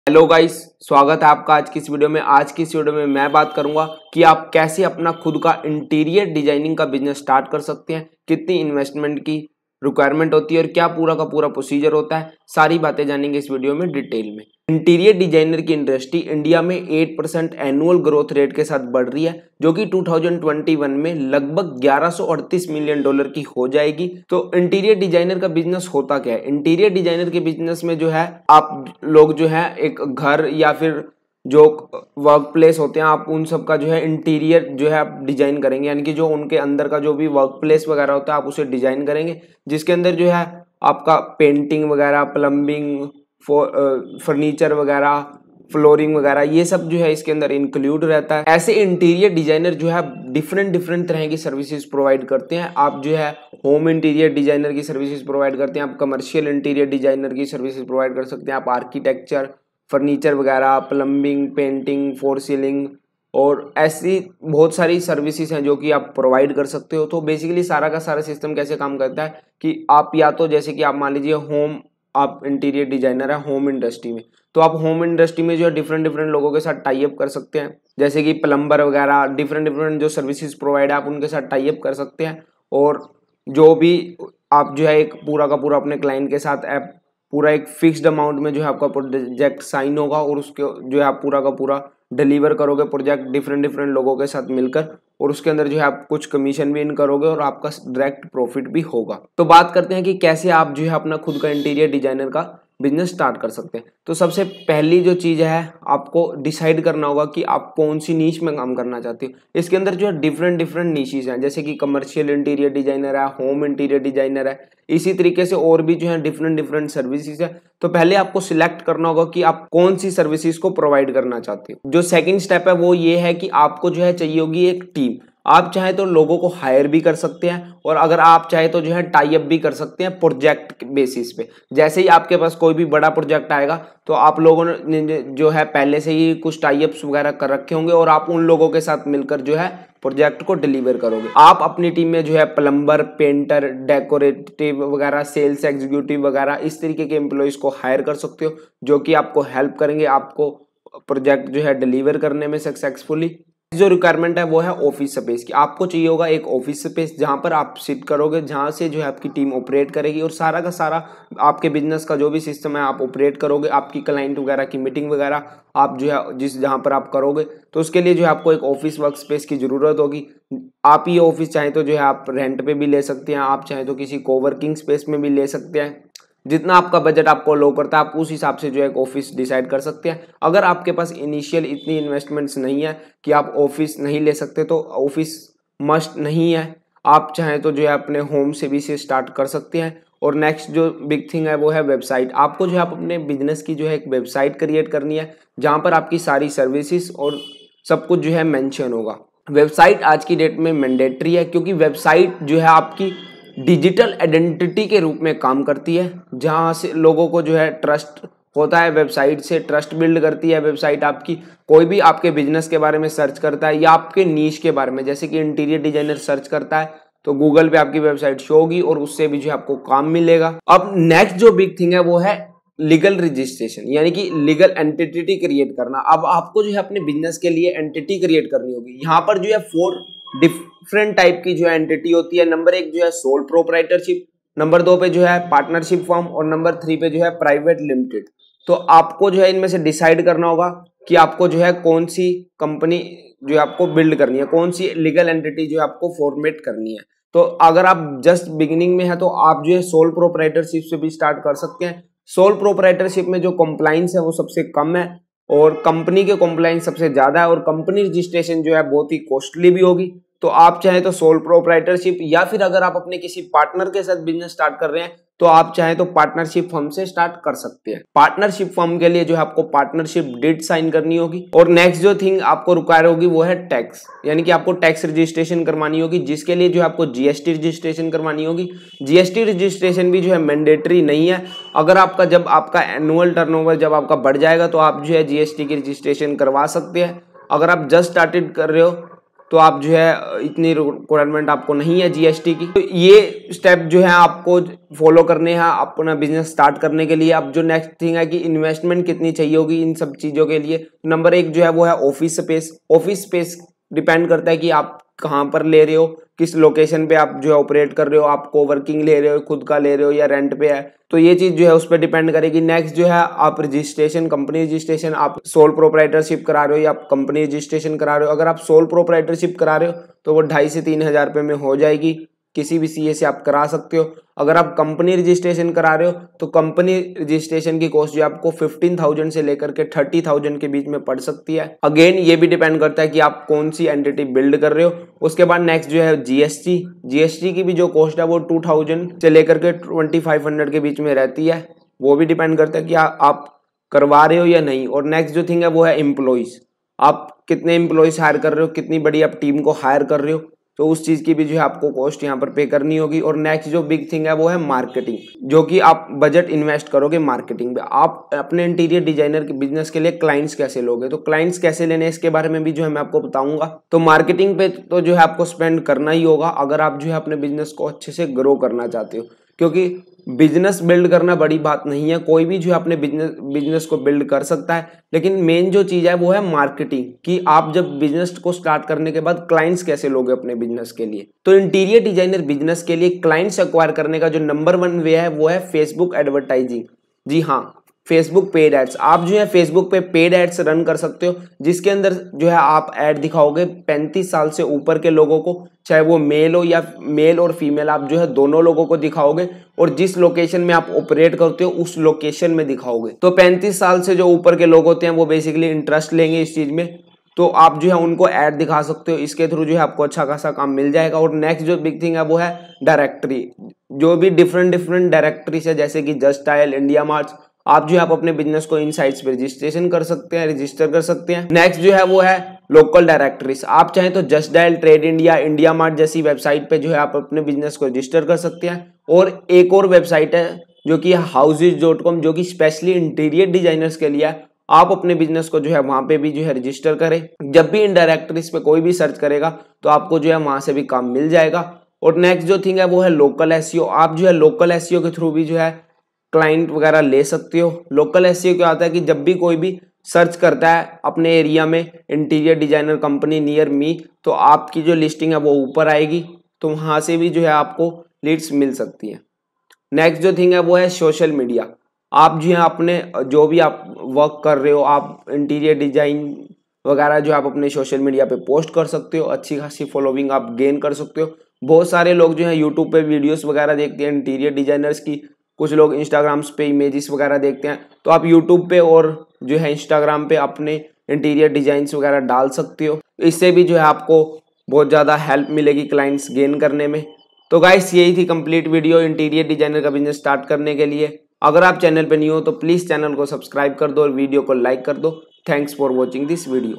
How to Start Interior Designer Business in India (Hindi) || Step By Step Guide
Watch this video to learn how to start Interior Designer Business With Zero Investment